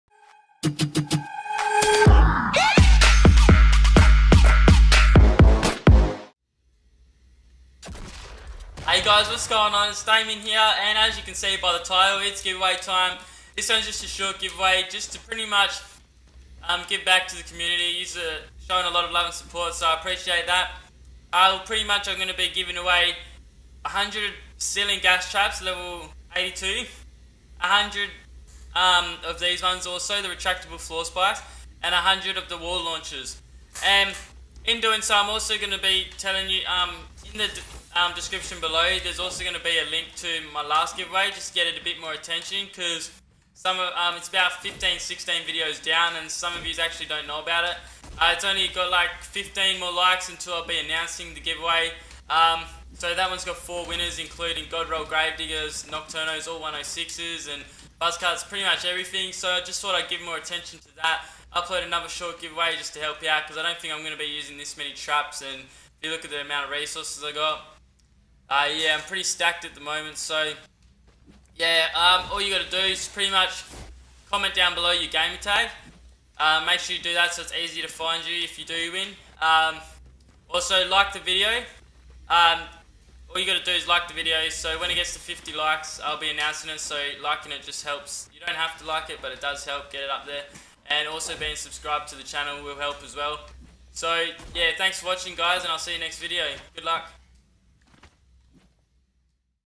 GIVE AWAY TIME (100 TRAPS - 3 WINNERS) All Platforms
PLEASE READ
So who comes up first when i use the comment picker will receive the wall punchers, second will get the gas traps and 3rd will get floor spikes!